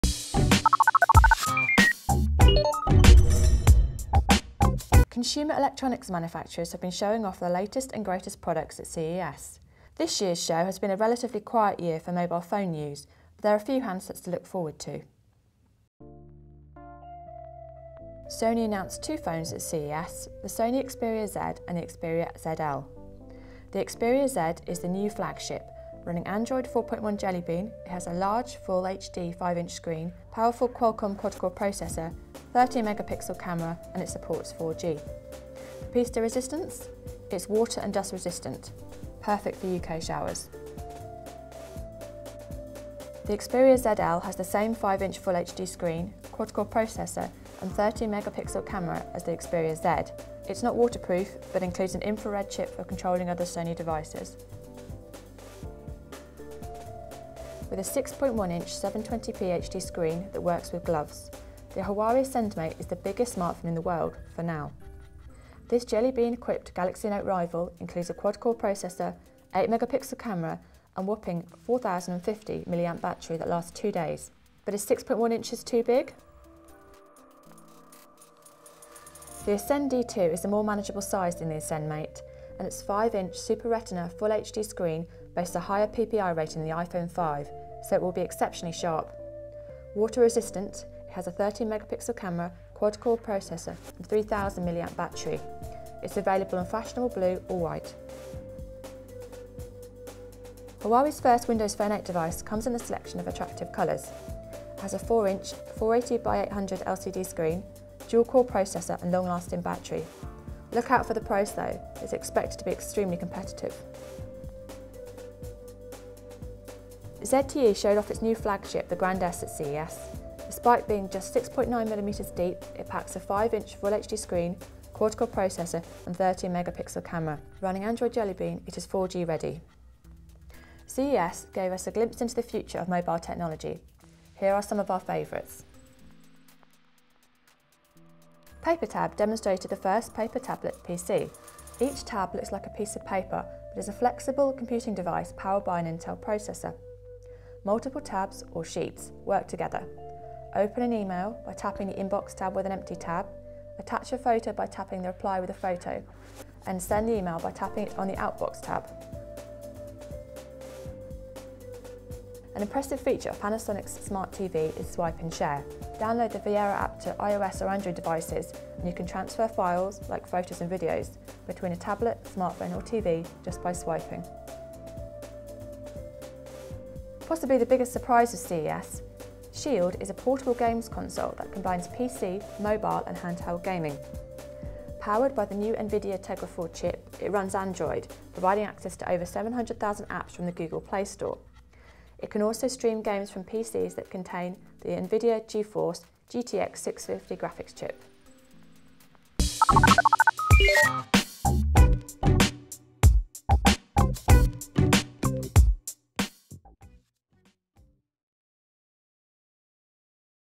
The Best Of CES 2013
CES takes place every year in Las Vegas, it's a chance for the consumer electronics industry to show off new products. Here's a look at some of the mobile highlights from Sony, Huawei and ZTE for 2013.